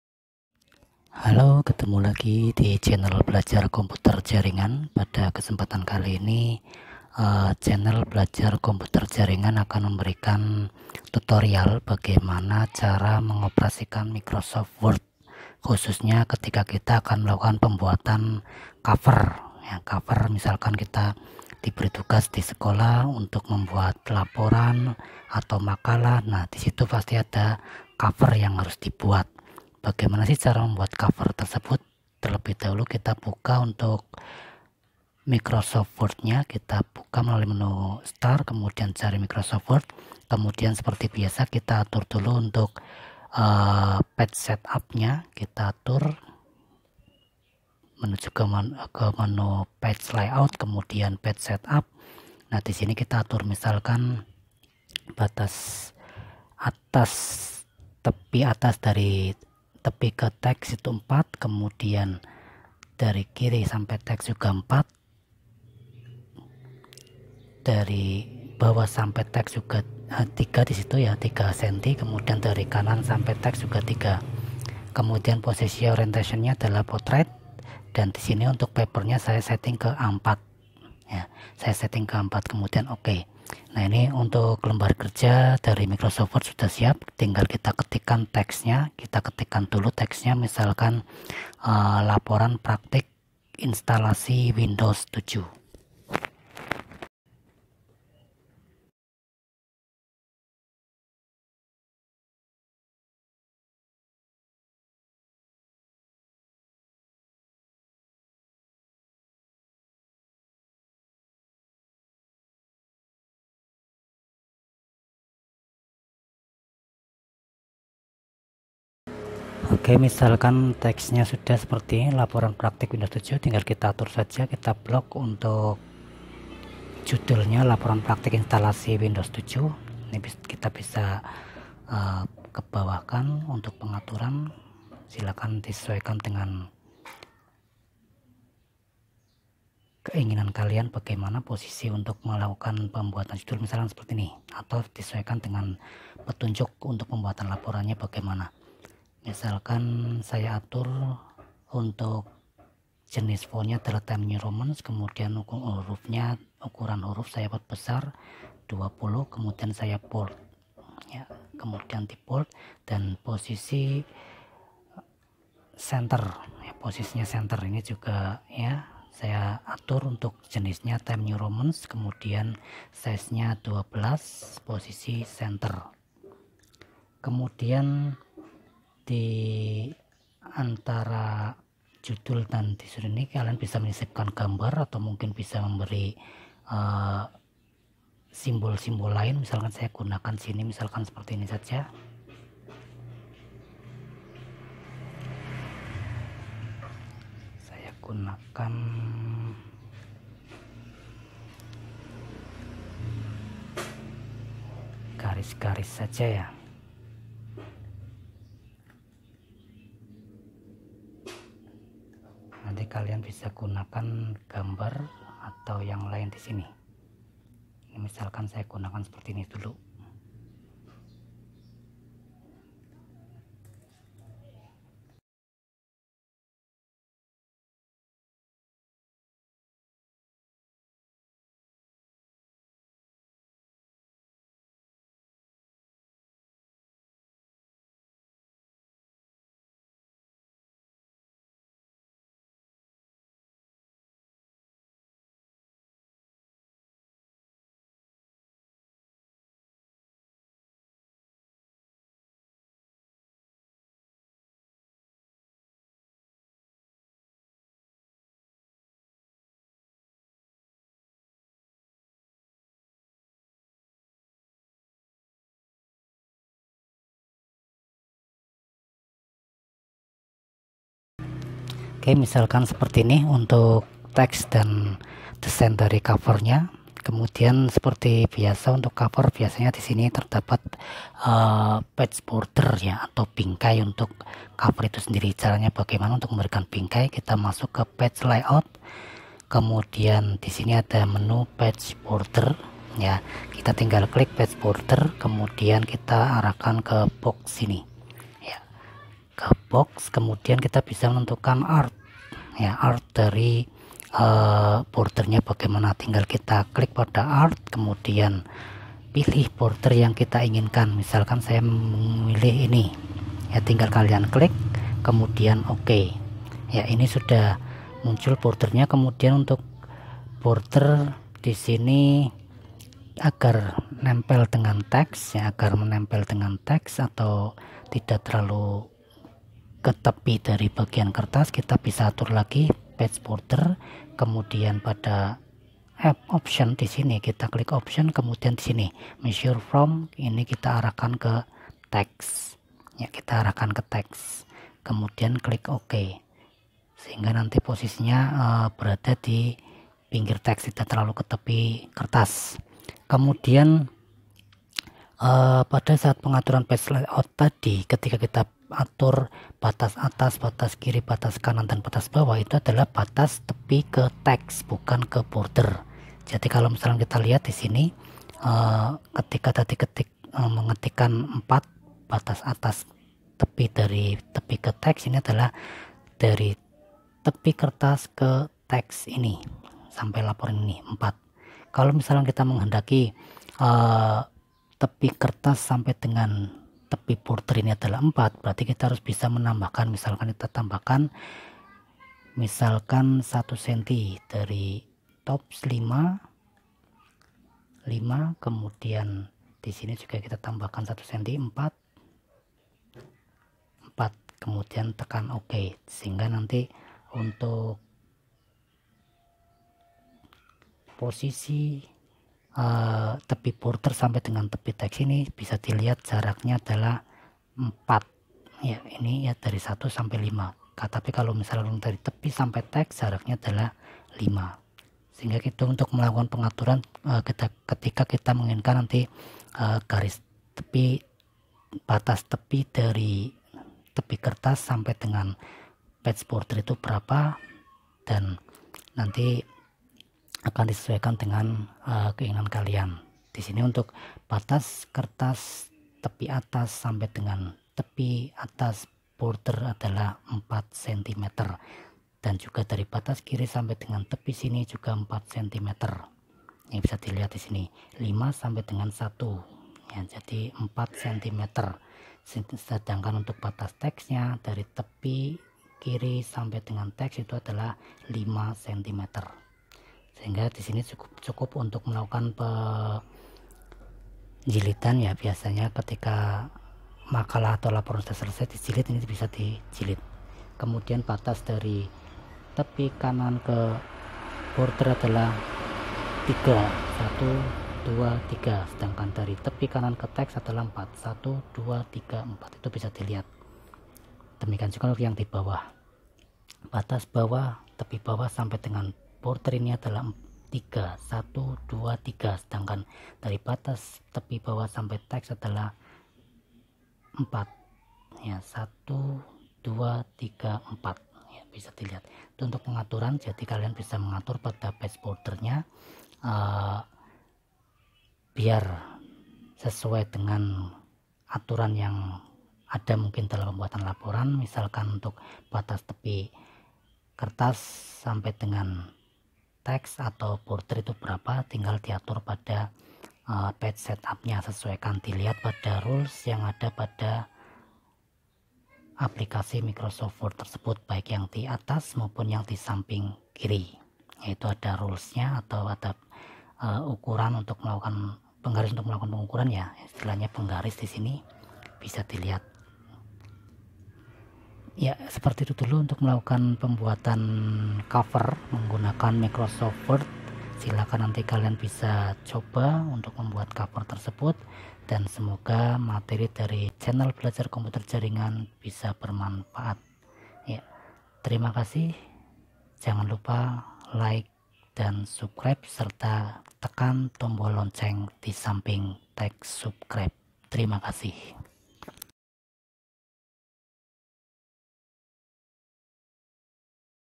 Membuat cover sederhana
Saat kalian membuat makalah,laporan tugas sekolah dan lainnya pasti ada yang namanya halaman cover yang dijadikan halaman sampul dan  di posisikan pada halaman pertama.
Bagaimana sih caranya untuk membuat cover tersebut?, Video ini menjelaskan langkah langkah dalam membuat cover sederhana menggunakan  microsoft word.
Silakan kalian bisa simak cara membuat cover sederhana ini.